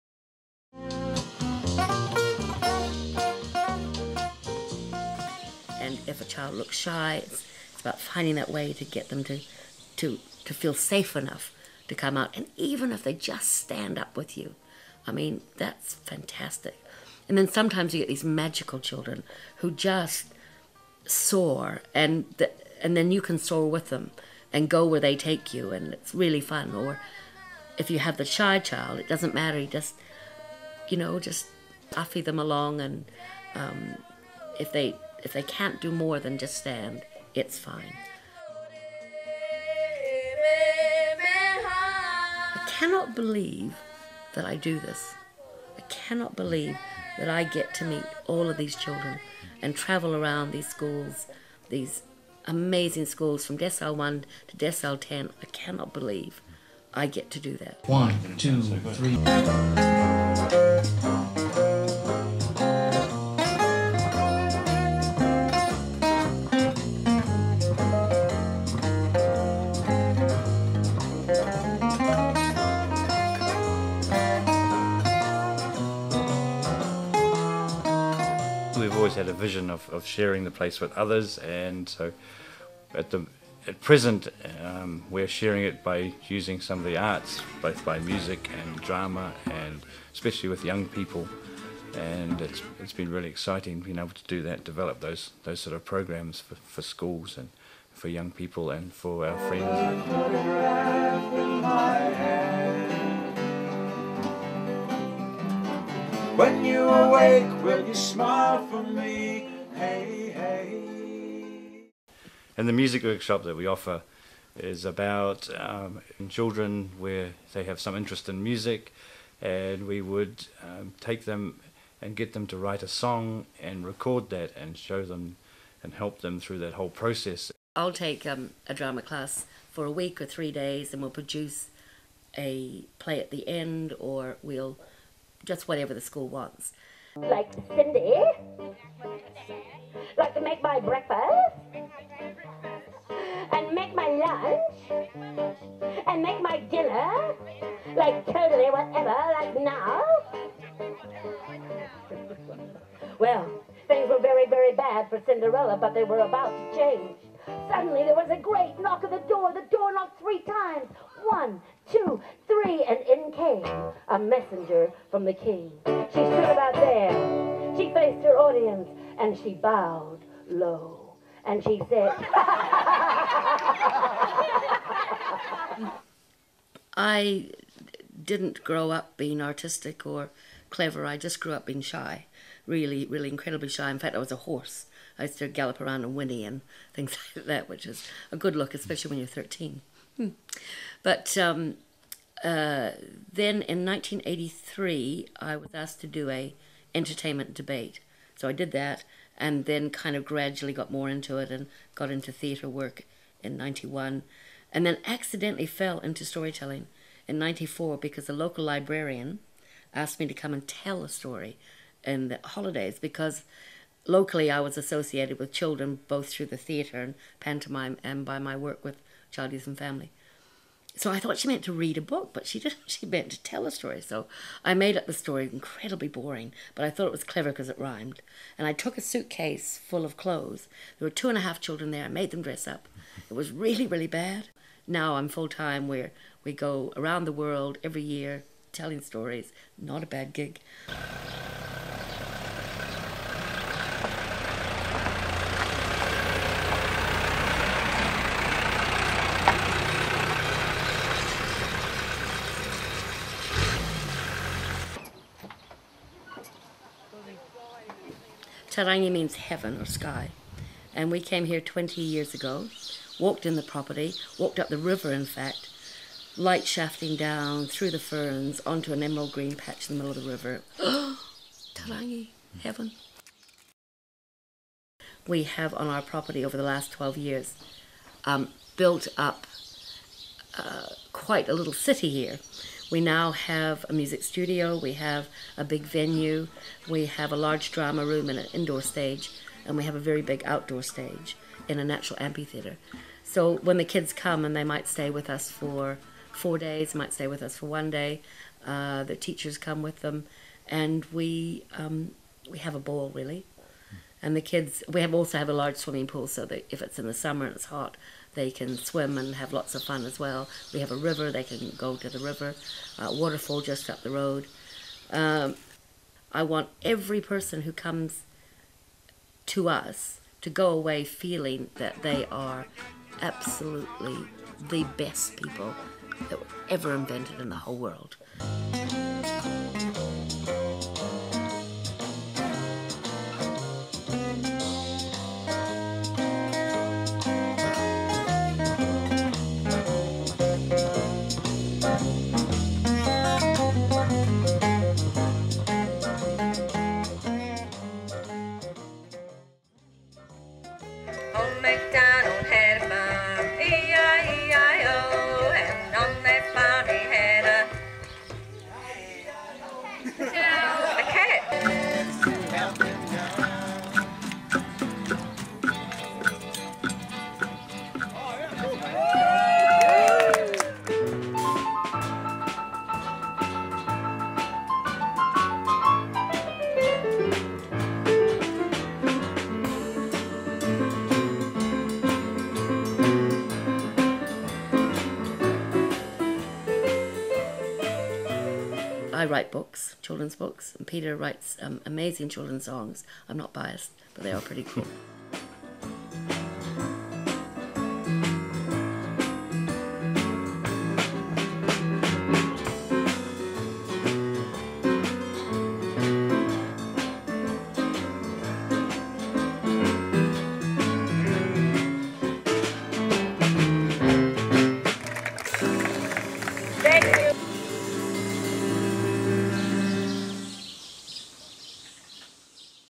Te Rangi ~ Mary Kippenberger & Peter Charlton-Jones (filmed by Press Go & Footpath Films
This video is an introduction to storyteller Mary Kippenberger and musican Peter Charlton-Jones. They live in a beautiful place called Te Rangi, a farm in the Central Hawkes Bay of New Zealand. They travel to schools all over the country and all over the world performing their colourful stories to children and adults. They also offer drama and music workshops. Their webiste is Filmed by Press Go Video and Footpath Films.